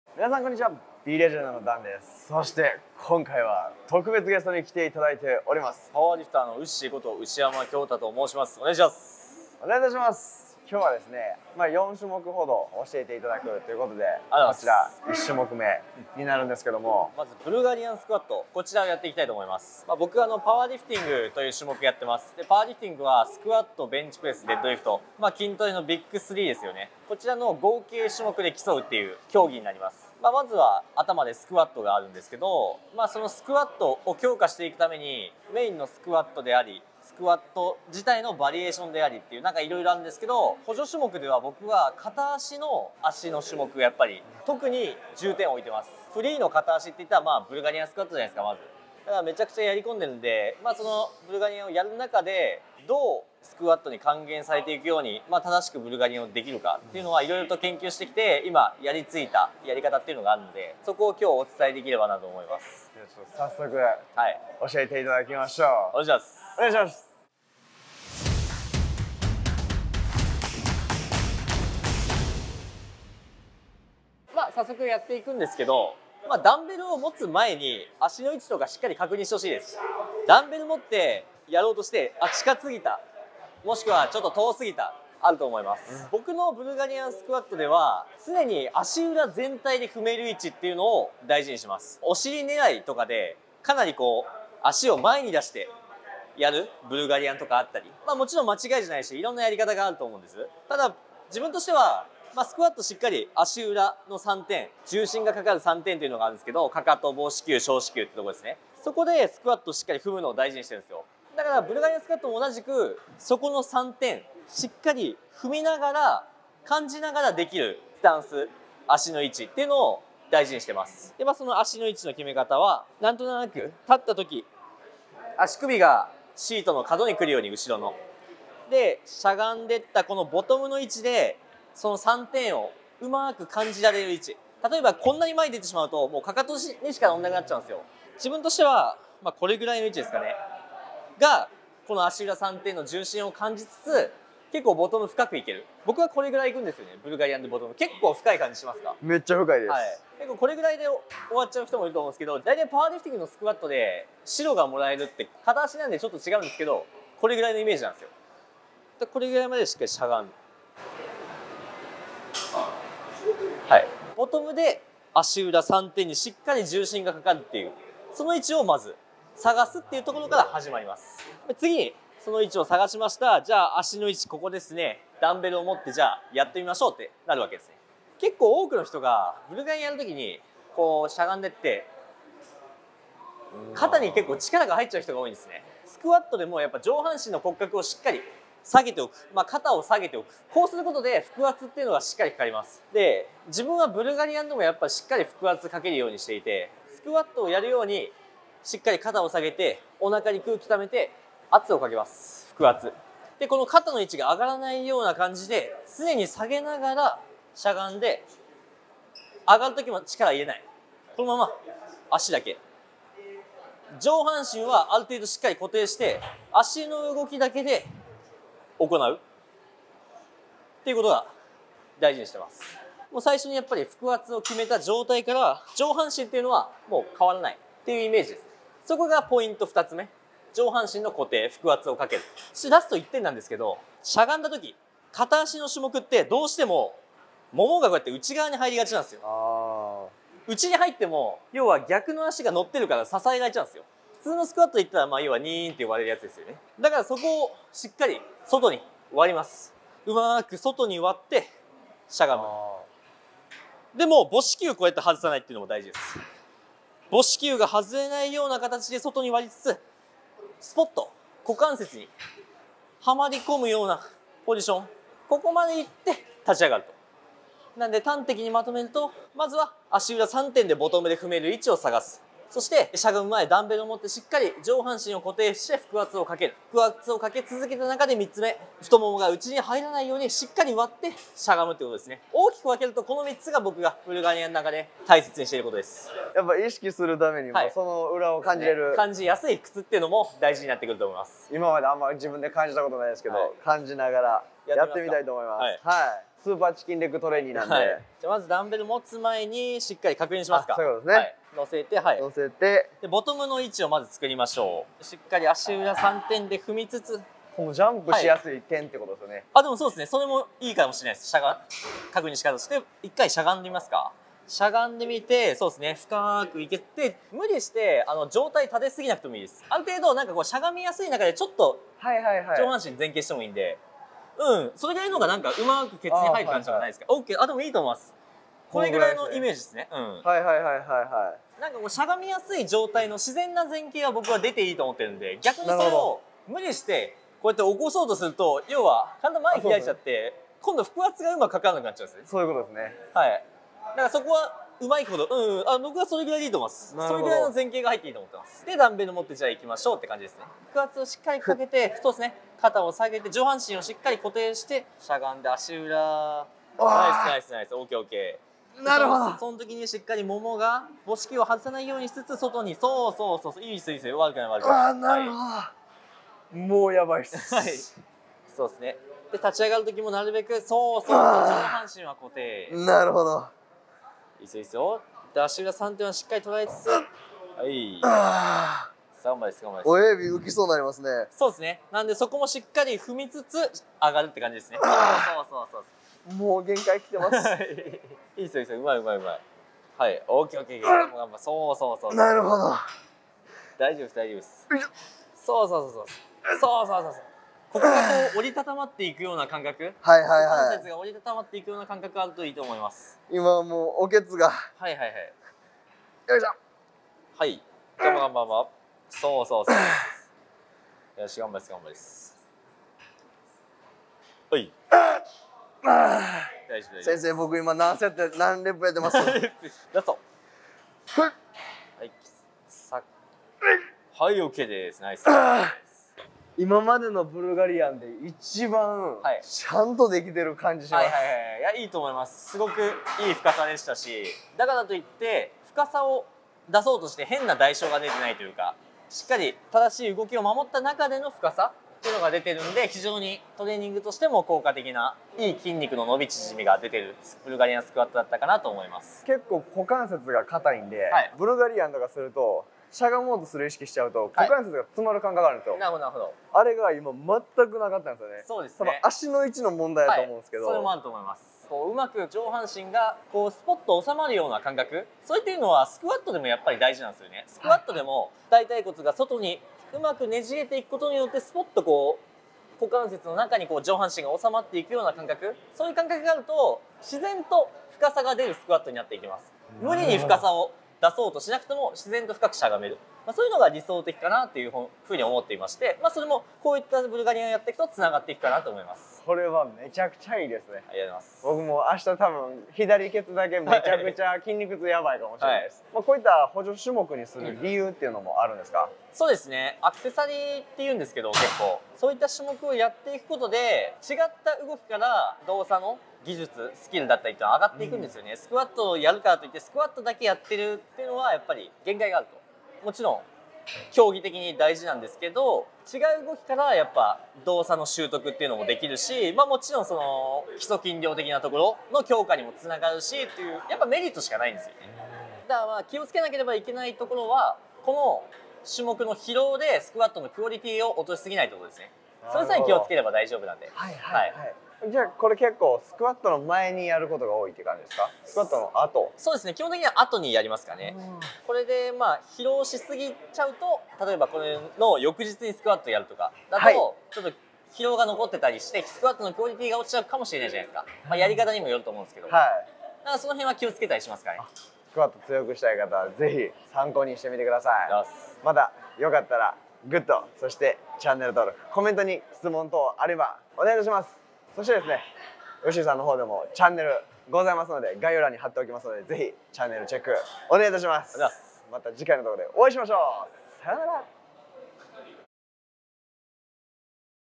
牛山恭太選手にスクワットの重量を伸ばす補助種目を聞いてみた！【ビーレジェンド プロテイン】
パワーチューブでもおなじみ「牛山恭太選手」にスクワットの重量を伸ばす補助種目を教えて頂きました！

■プロテインやサプリメントがお得にご購入できるタイムセール開催中！
■大人女性の燃焼トレーニング【OIC公式】